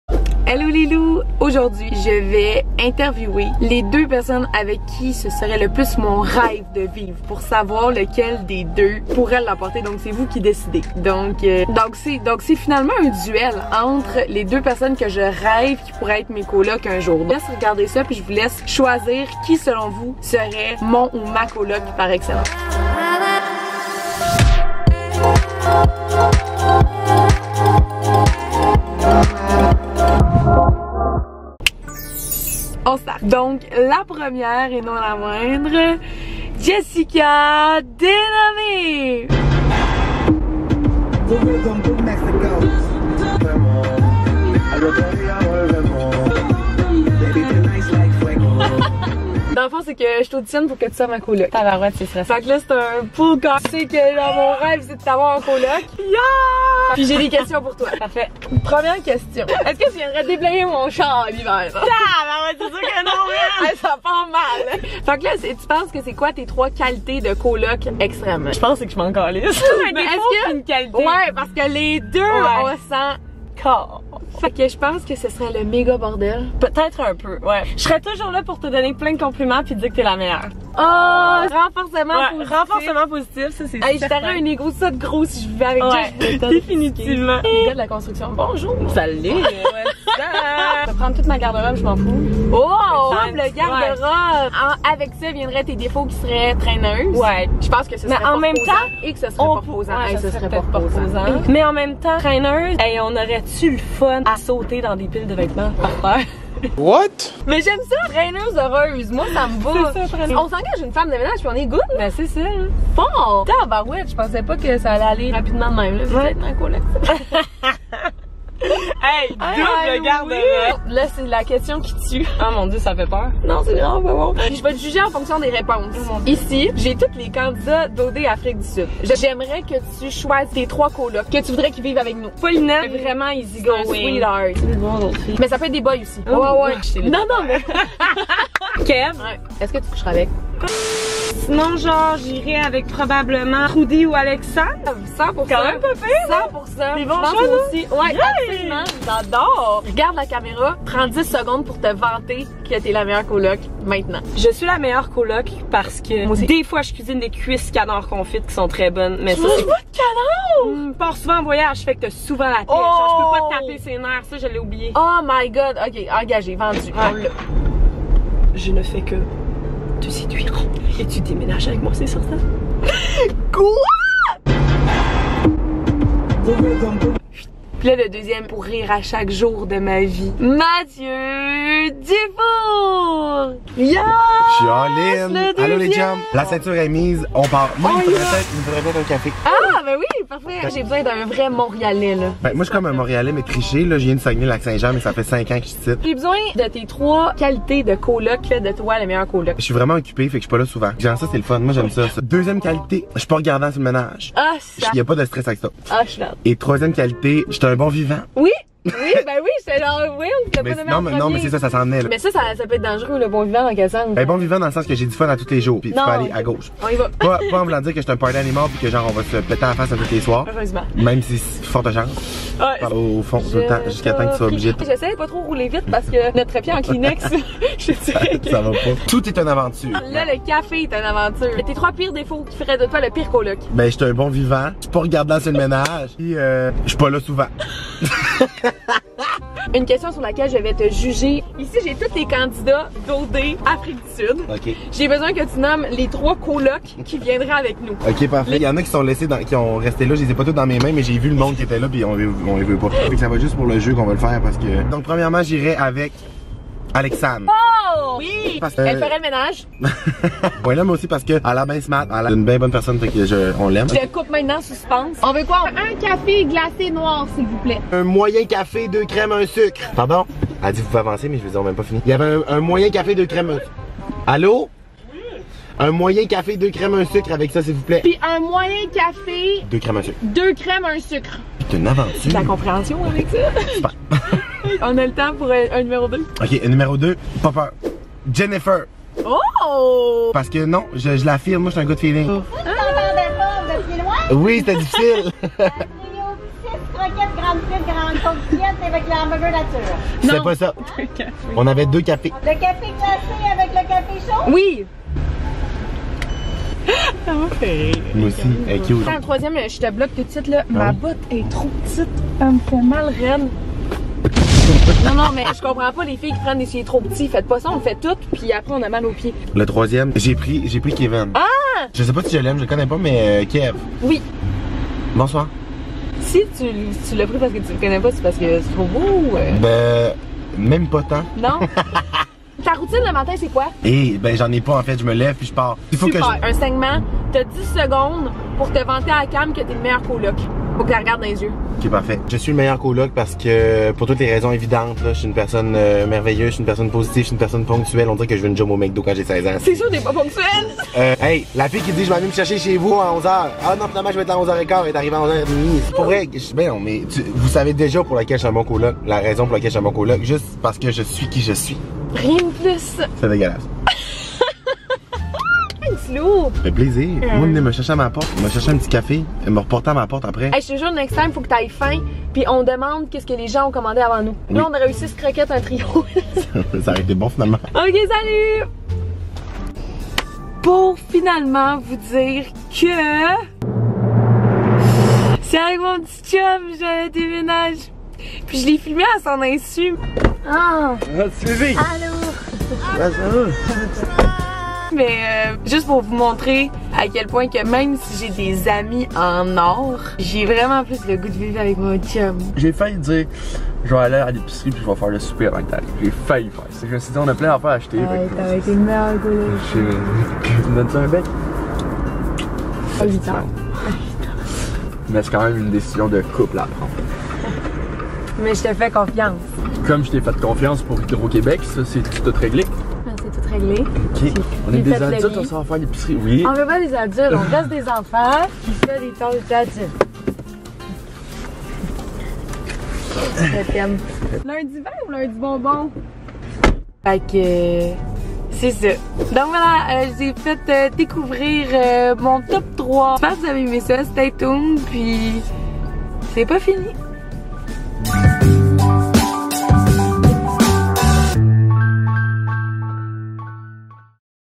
Duel, avec qui je vais vivre ? JESSIKA vs MATHDUFF
Juste du gros fun. Les voyages c'est en 2020, en attendant on s'amuse.
Merci d'être la vous êtes malade.

LE MEILLEUR COMME D'HAB monteur : @Alexthelucky